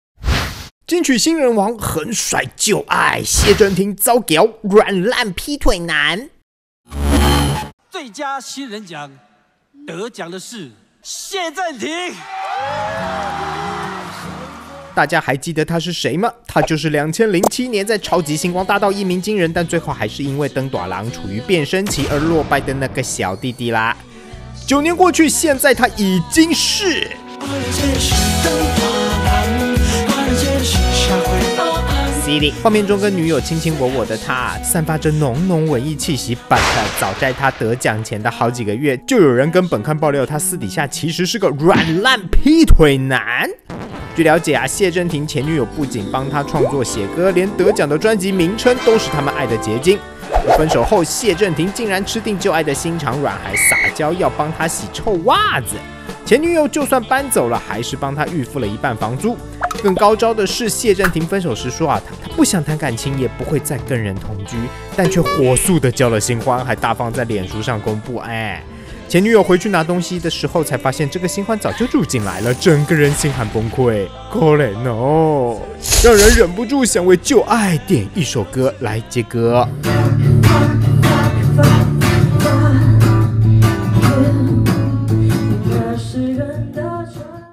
【台灣壹週刊】金曲新人王遭控軟爛劈腿 狠甩前女友
今年金曲獎奪下最佳新人獎的謝震廷，他的歌聲算是療癒，但感情生活複雜。本刊接到讀者爆料，說他一邊狠甩掉前女友，一邊已經迫不及待交了新歡。同時本刊也多次捕獲謝震廷跟新歡約會的畫面，而且小倆口更高調在臉書上宣佈穩定交往中。